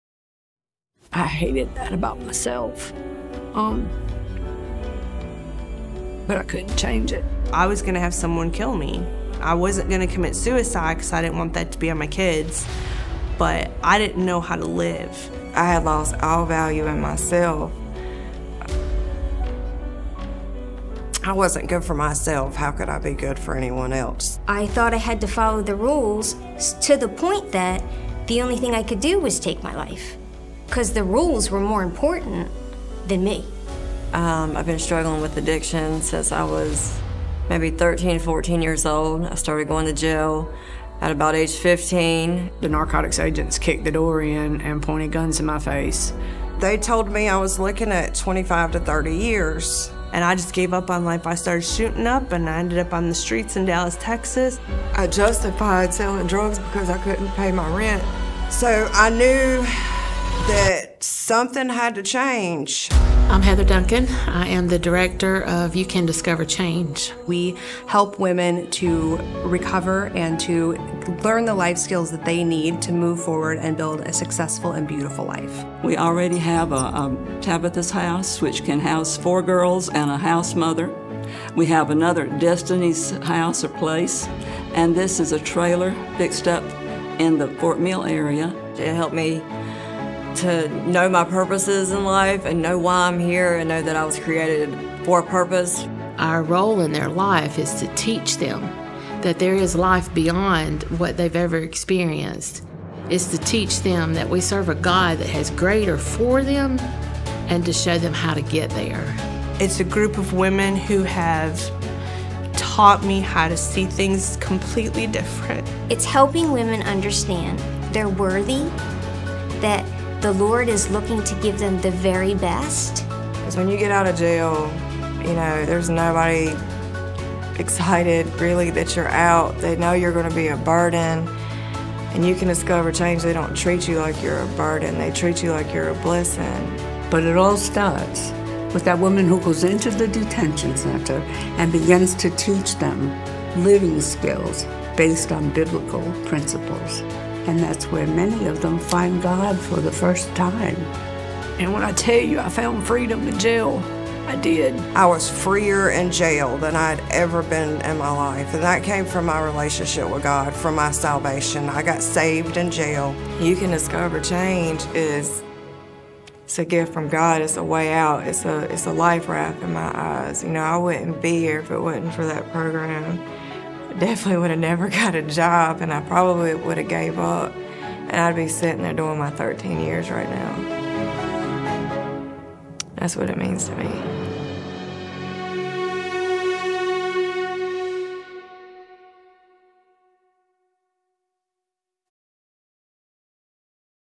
You Can Discover Change
God is Still a God of Restoration and Healing No situation is too far gone for God to restore.